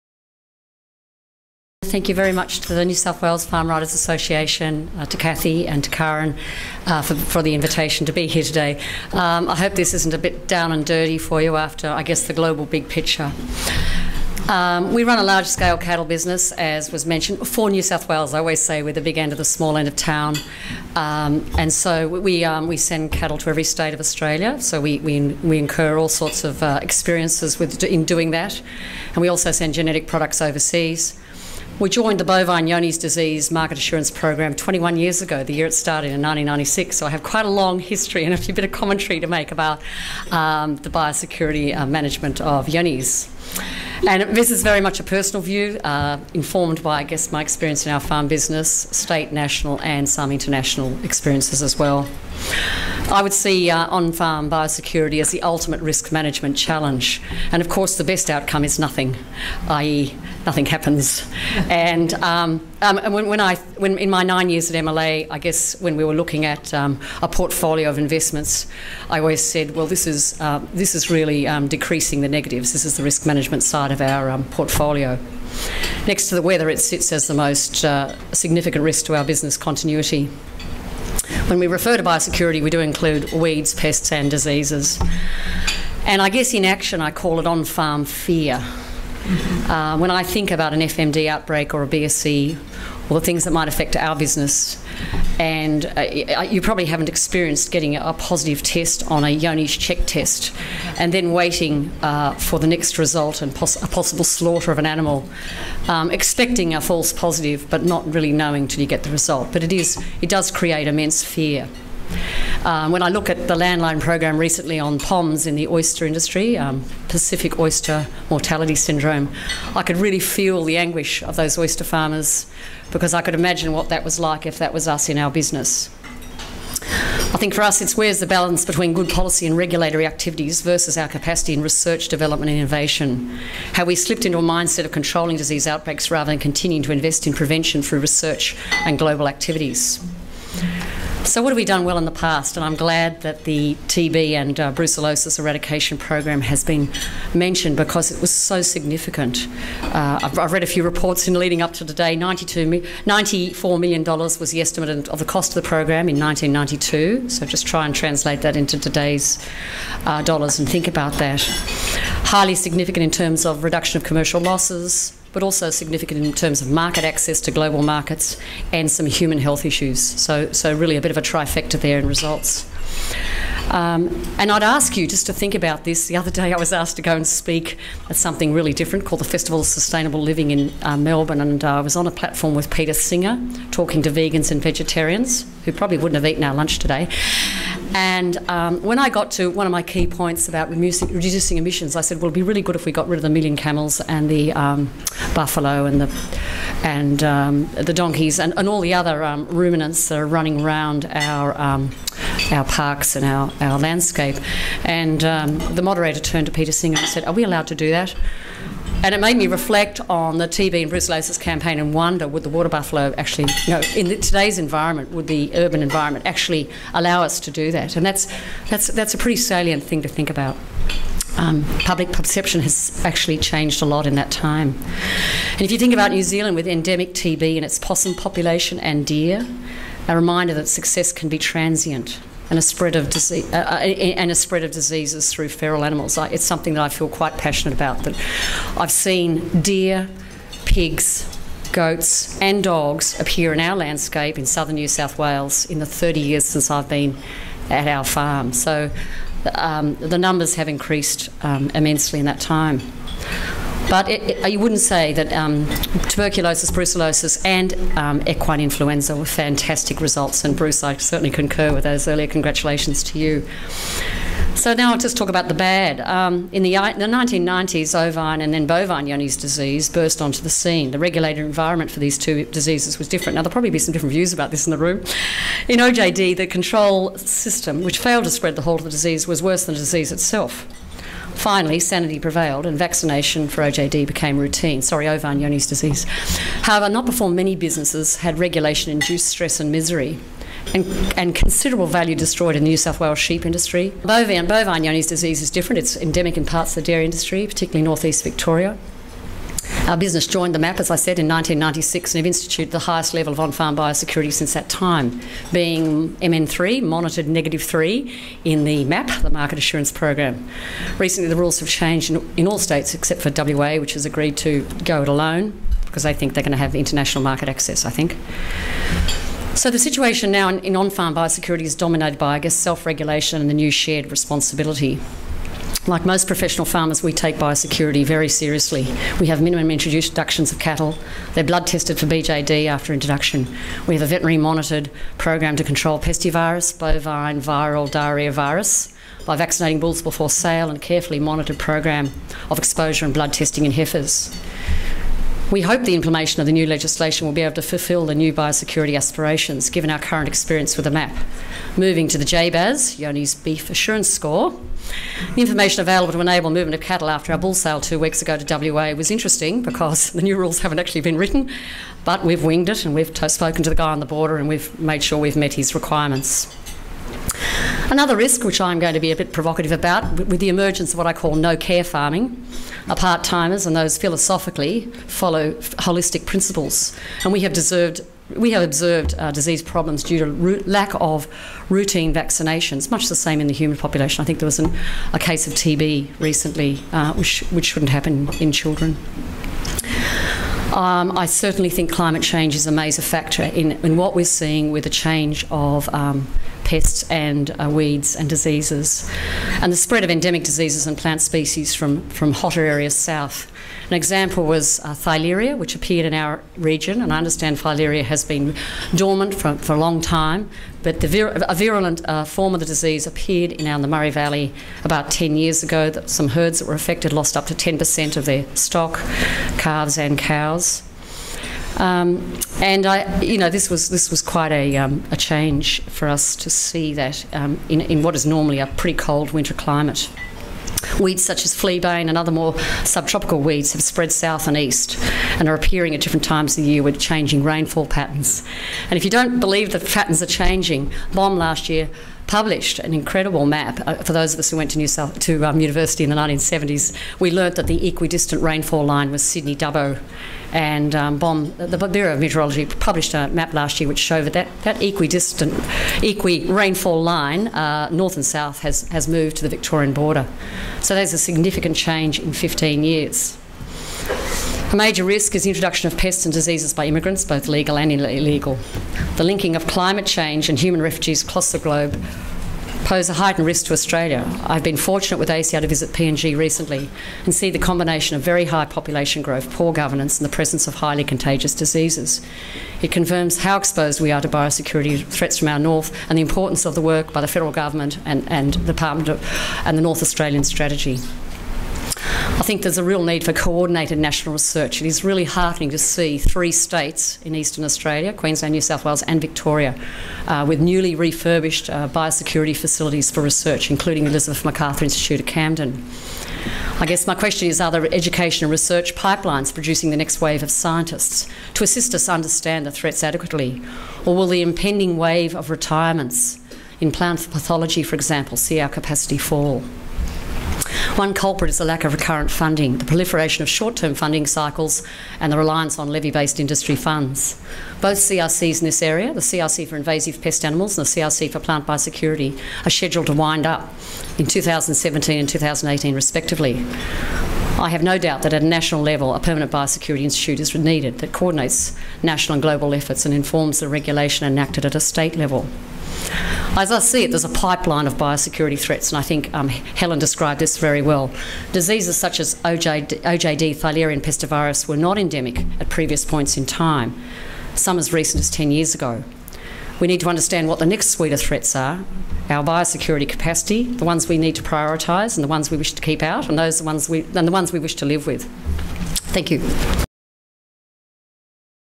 Lucinda Corrigan - Biosecurity: National Strategies and International Challenges
Lucinda Corrigan is the Director of Rennylea Pastoral Company, a progressive Angus genetics business running 3,000 cattle, located in the Murray Valley east of Albury.

She chairs the NSW Primary Industries Minister's Advisory Council and the Advisory Committee of the Graham Centre for Agricultural Innovation.  She has been a Director of four cooperative research centres and is a commissioner of the Australian Centre for International Agricultural Research.

She has been a Director of Meat & Livestock Australia and also worked in the fibre industry.